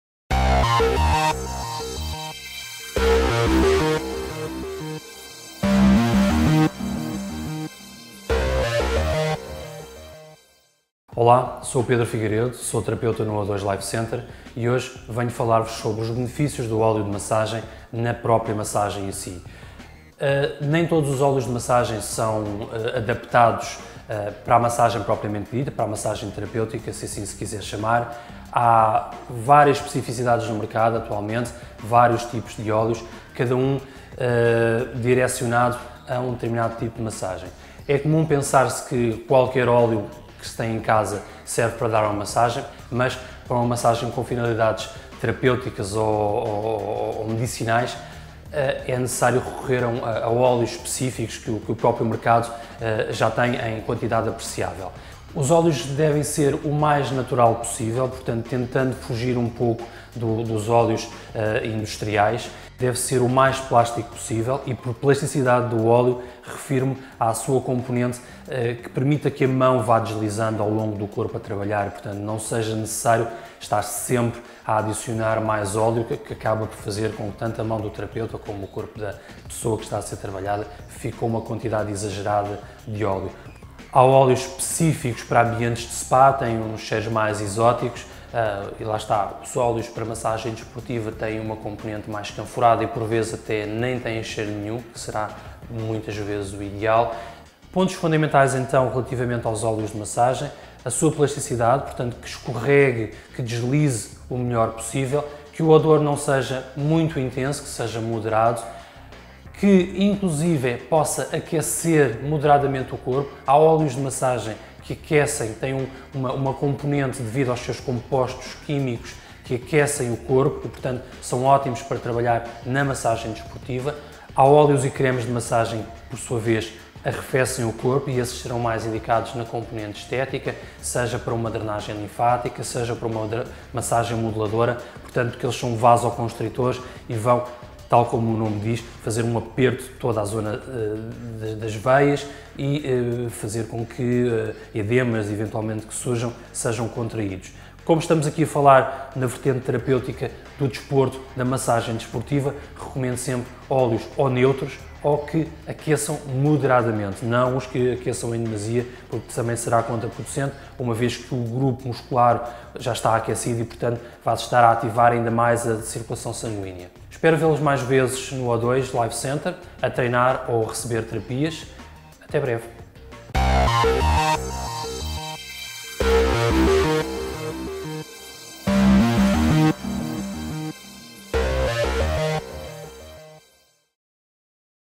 A importância dos óleos de massagem
Nas massagens terapêuticas, os óleos não servem para "besuntar" as nossas mãos, também têm a sua importância. No entanto, nem todos os óleos servem para o fim. Saiba quais são os "segredos" dos óleos para termos uma melhor recuperação.